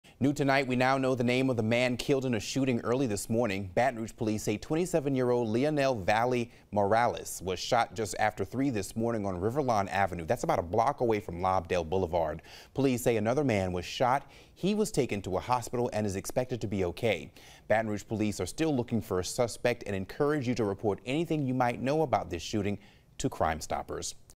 1 killed, 1 injured in early morning shooting in Baton Rouge
Police are investigating a shooting that left one person dead and another one injured early Sunday morning, May 26.